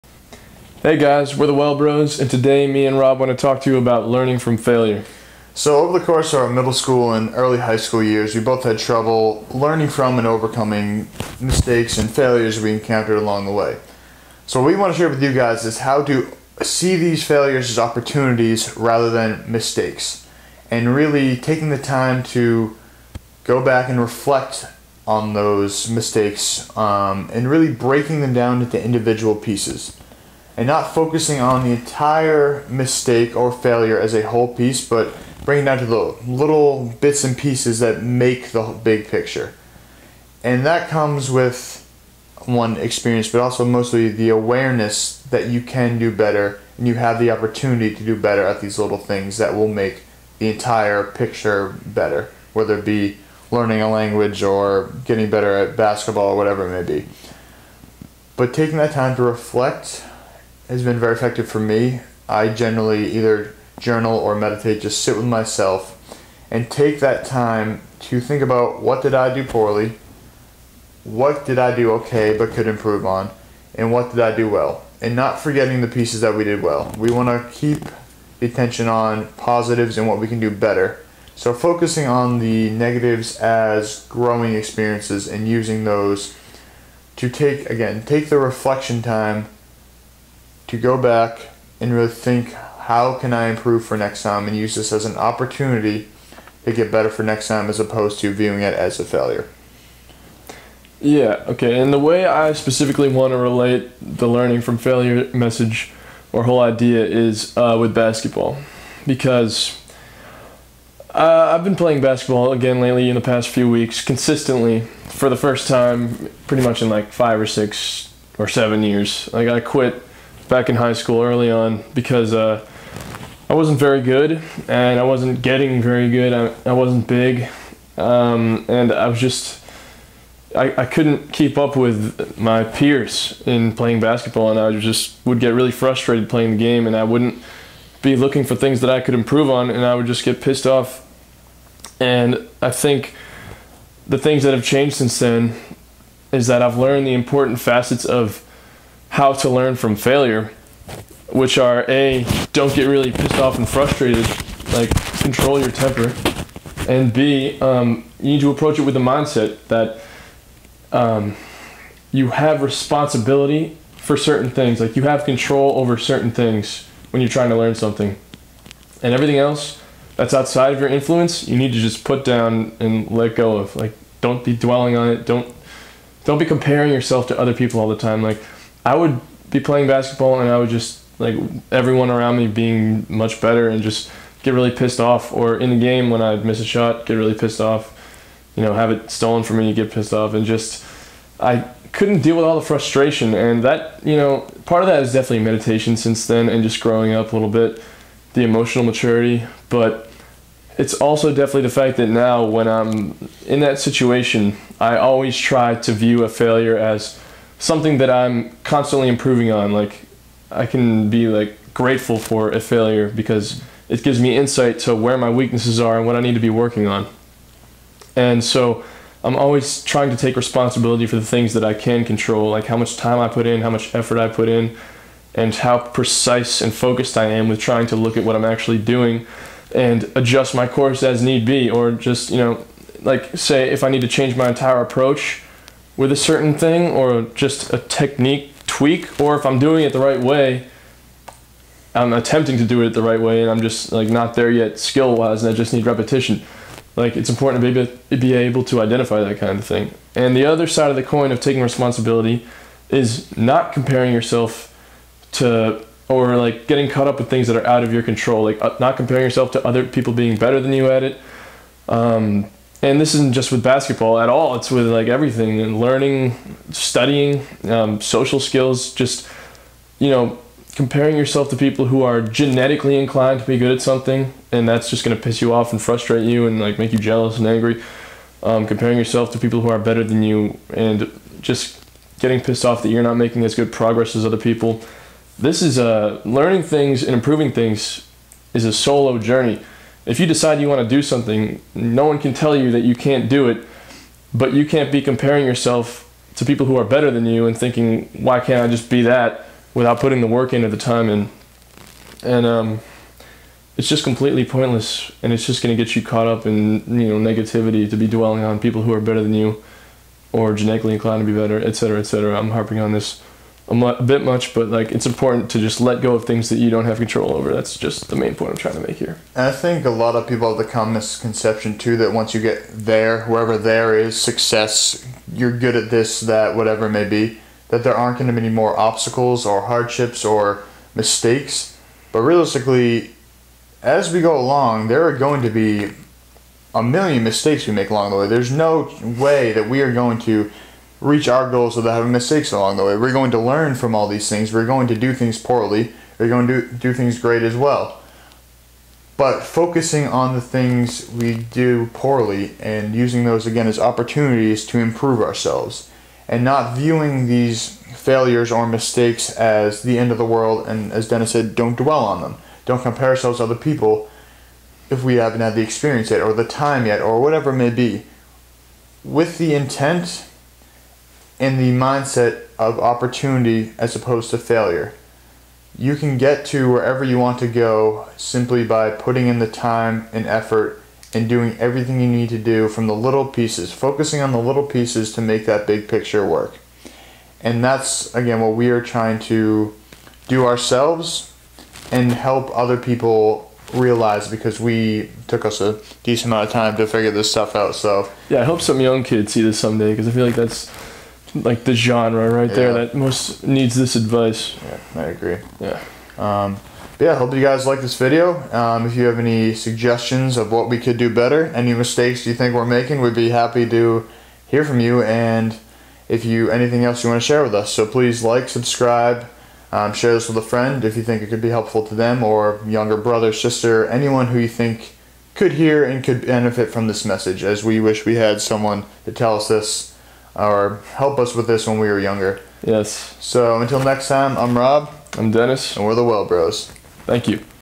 [WB] Learning From Failure
In this video, the Well Bros discuss how to learn from mistakes.  Included are different strategies and mindsets for moving forward to be prepared for the obstacles and hardships that the future may bring.